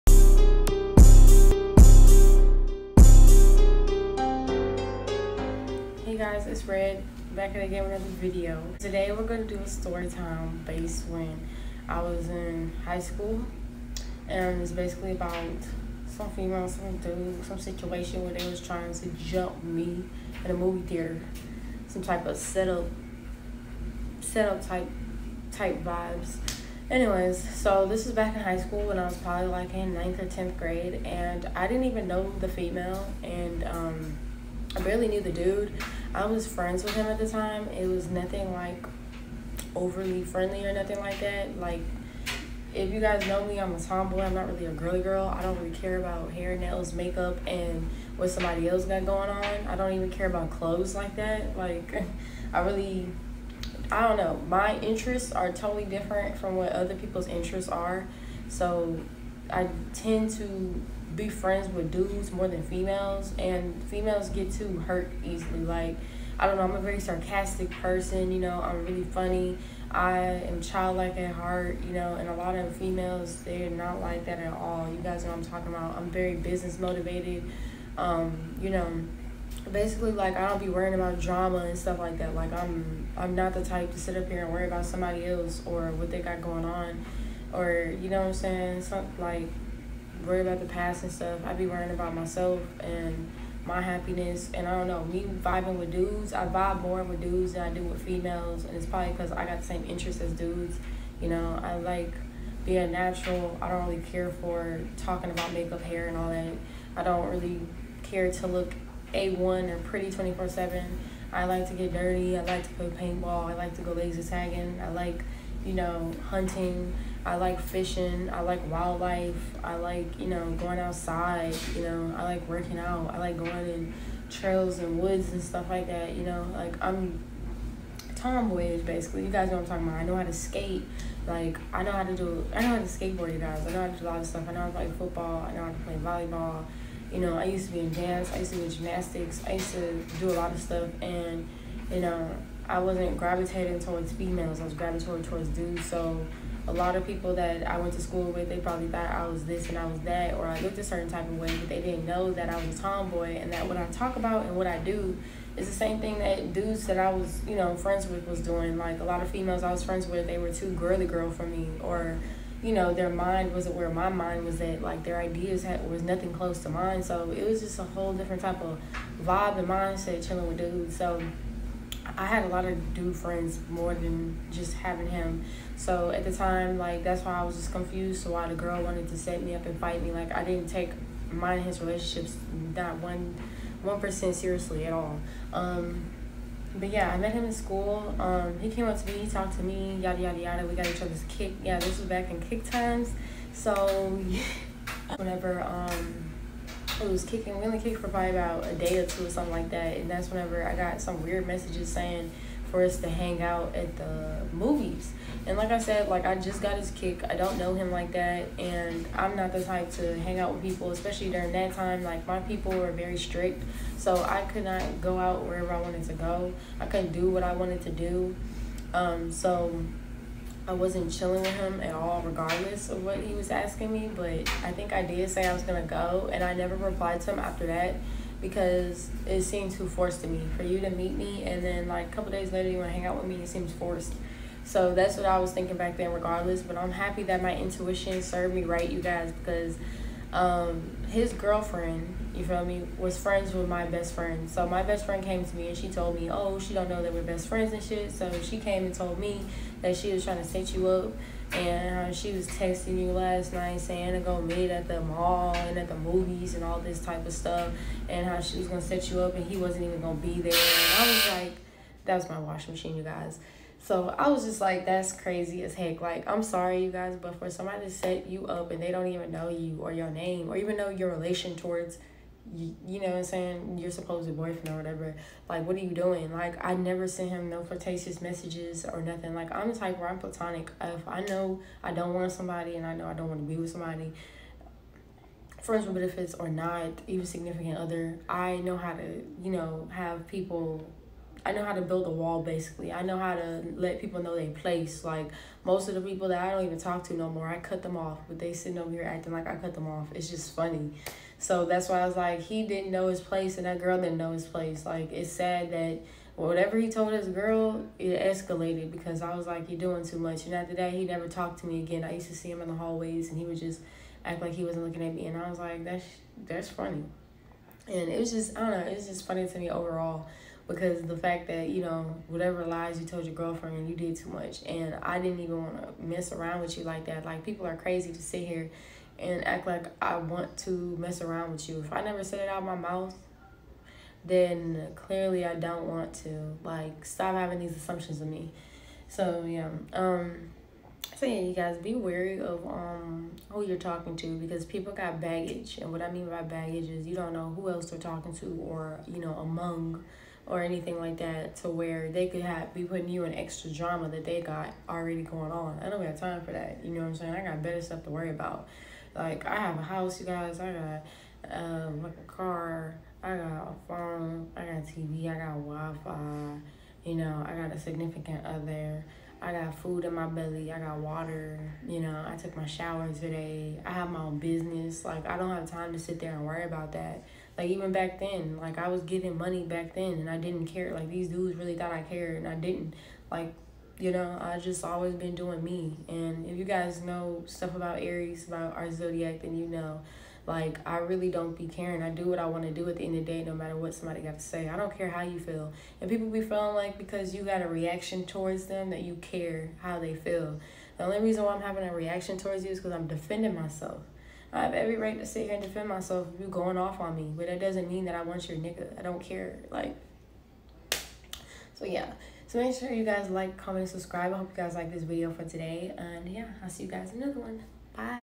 SHE TRIED TO SET ME UP!? 🫢| STORYTIME
Hey hunnid familyyyyy its red with another storytime (: i remember this being the funniest situation i was ever dealing with🤣🤣 and i wanted to share this with you guys !